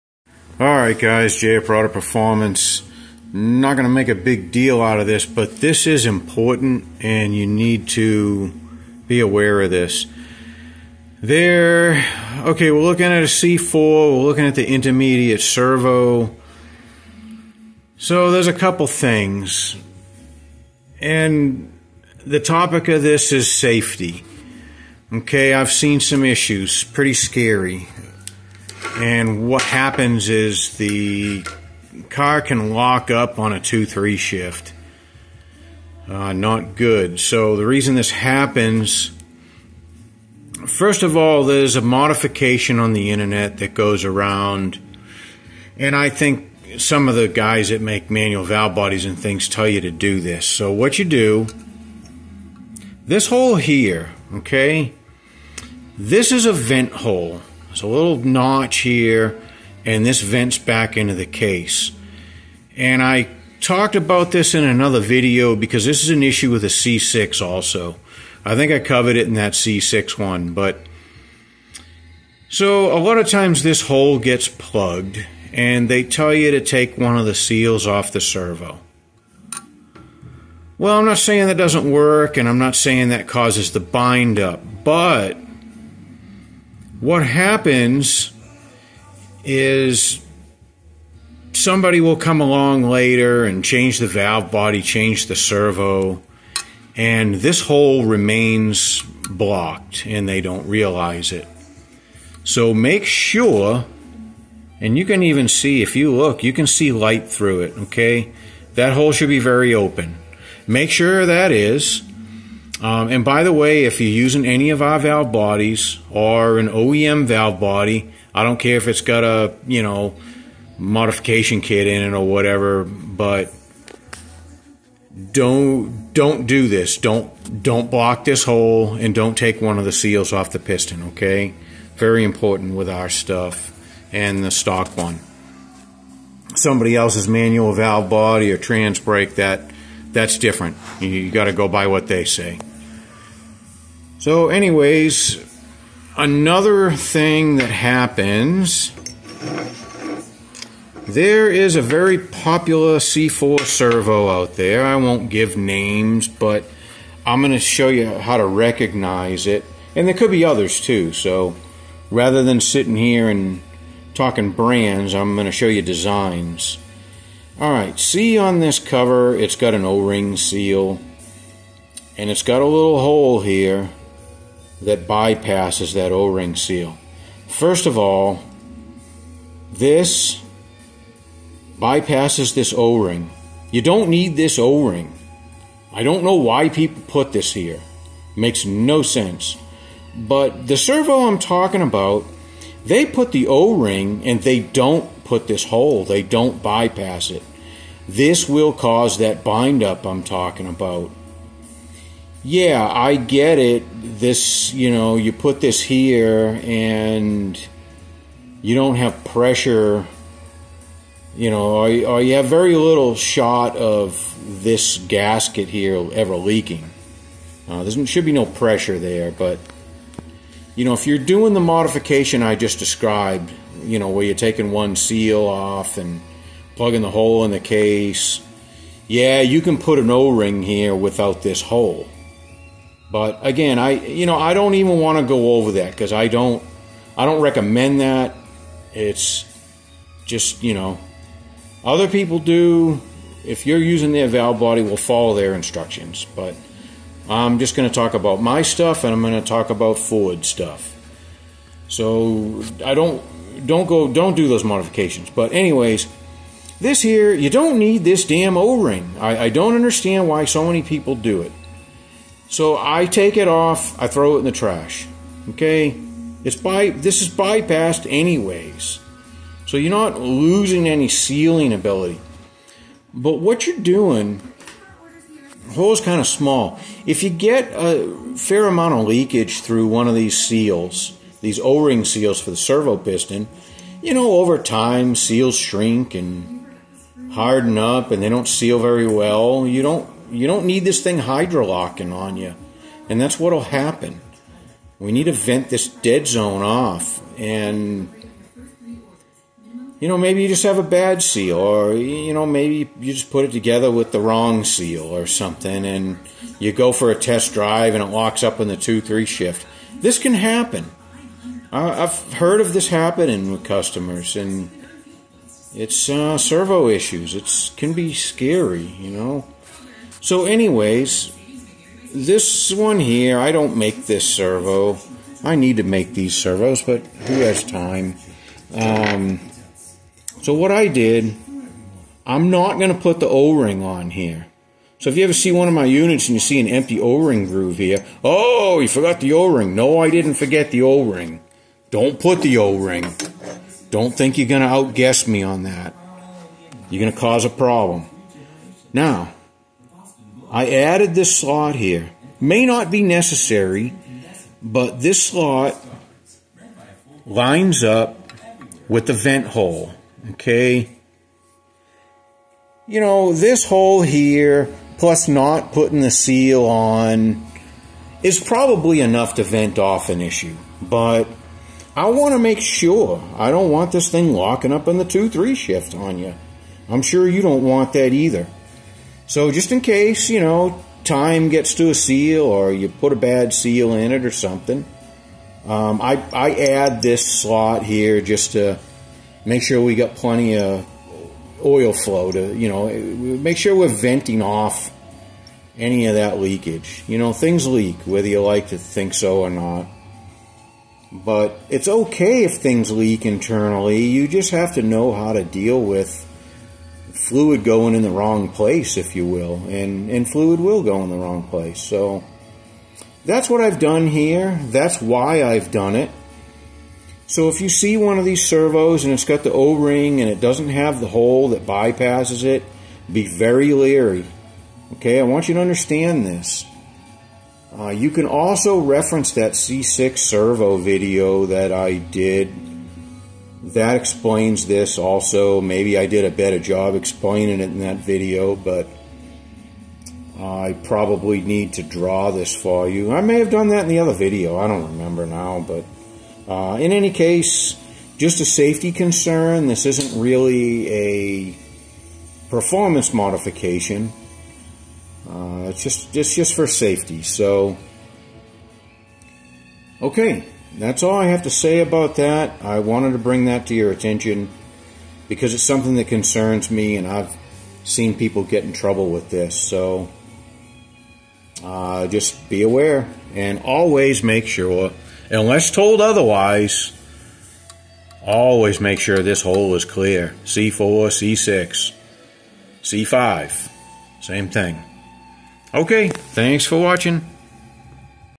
Ford C4 Transmission Servo Modifications and Safety Concerns
In this video Jay Robarge of Broader Performance will go over some common intermediate servo designs, modifications as well as safety concerns when making modifications to the servo on Ford C4 transmissions.